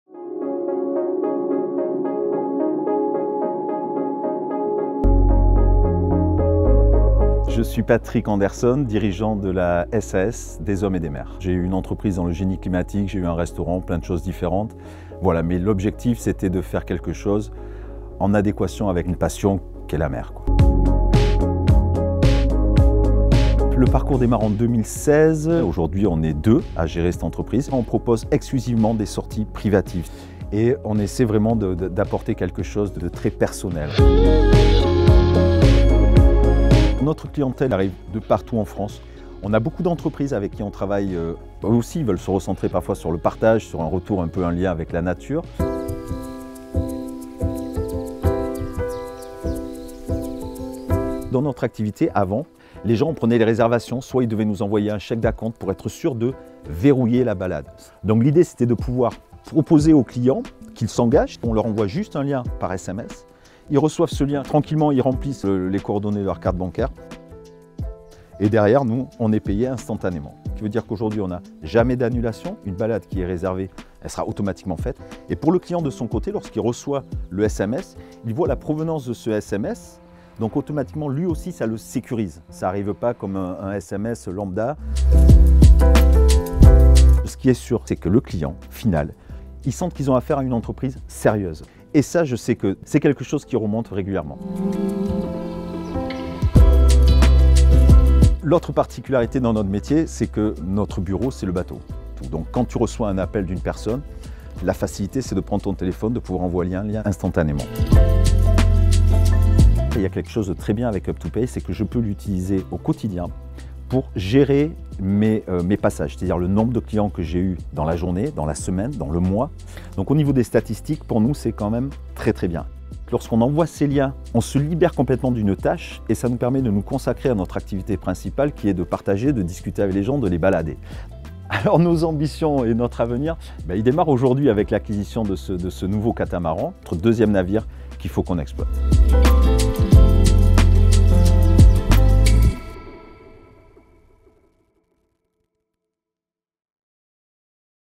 C’est dans ma boîte : Patrick et le paiement par lien
Découvrez comment Patrick Anderson, gérant de la société de tourisme maritime Des Hommes et des Mers, développe son activité en simplifiant la réservation de ses clients, directement sur son catamaran, grâce à la solution d’encaissement à distance Up2pay Par lien.